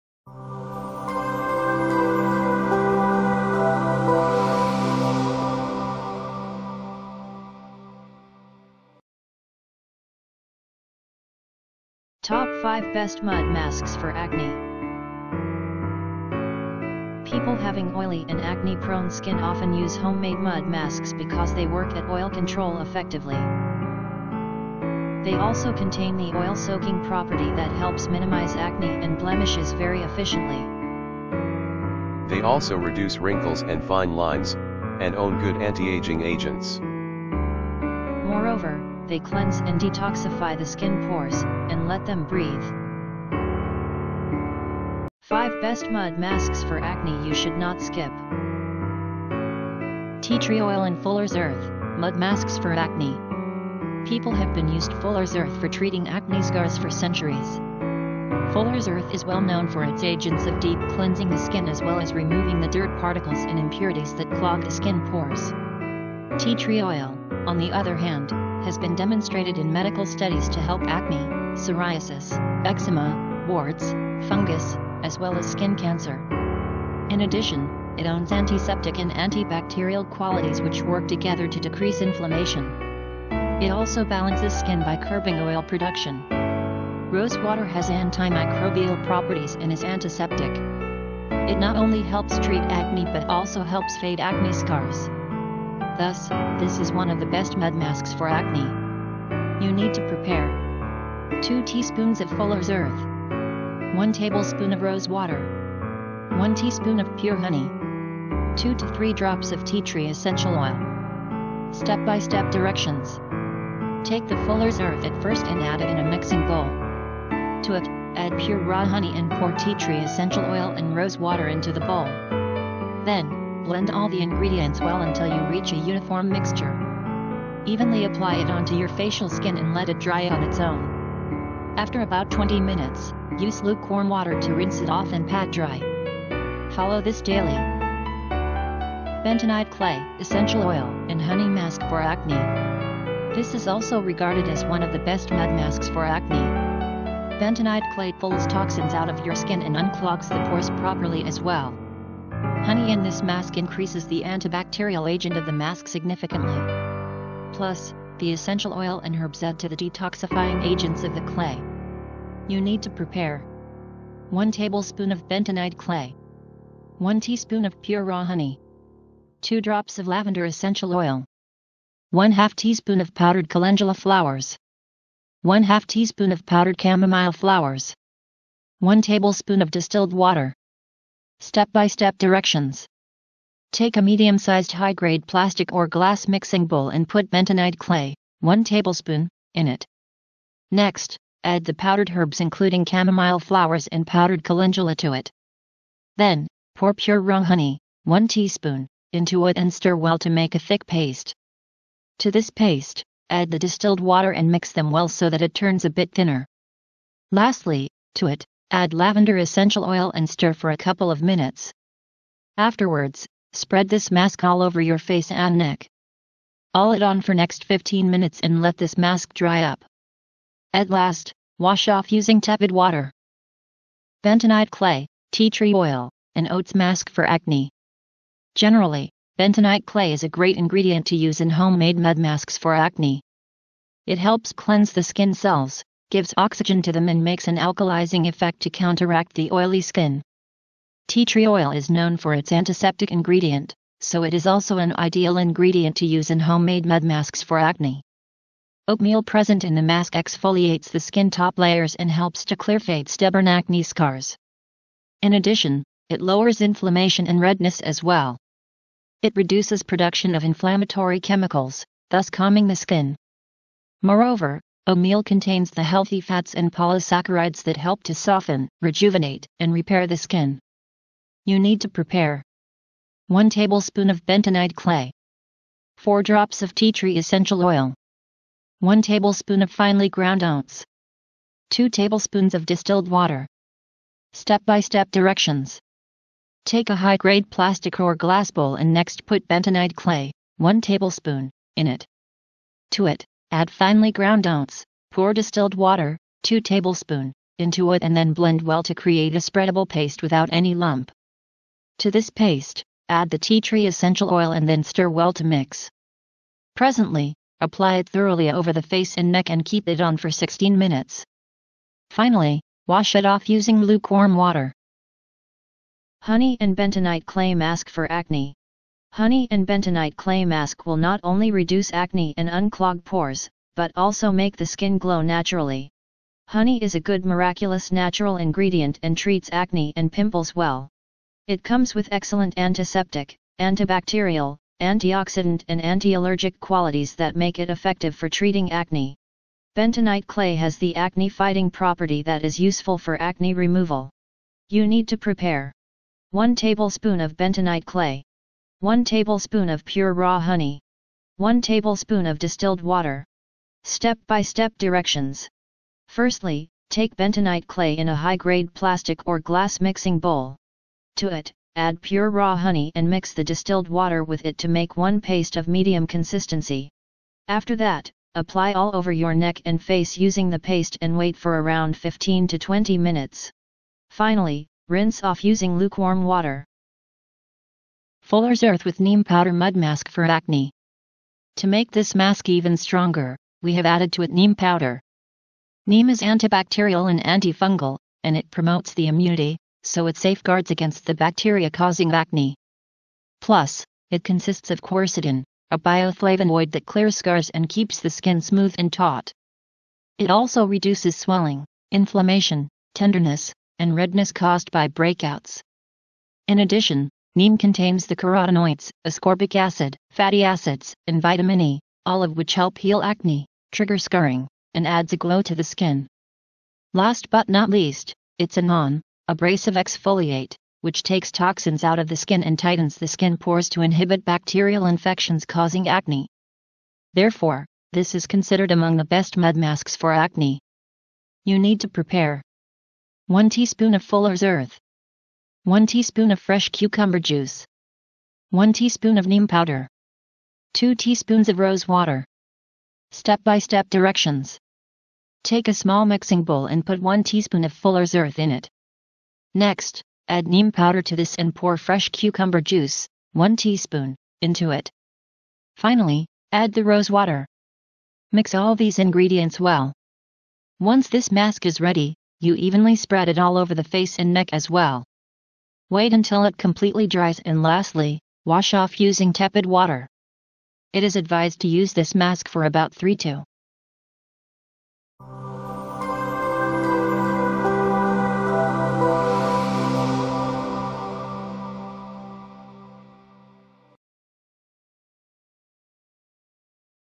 Top 5 Best Mud Masks For Acne
People having oily and acne-prone skin often use homemade mud masks because they work at oil-control effectively.  They also contain the oil-soaking property that helps minimize acne and blemishes very efficiently. They also reduce wrinkles and fine lines, and own good anti-ageing agents. Moreover, they cleanse and detoxify the skin pores, and let them breathe.

1. Tea Tree Oil And Fuller’s Earth – Mud Masks For Acne
2. Honey And Bentonite Clay Mask For Acne
3. Fuller’s Earth With Neem Powder Mud Mask For Acne
4. Bentonite Clay, Tea Tree Oil, And Oats Mask For Acne
5. Bentonite Clay, Essential Oil, And Honey Mask For Acne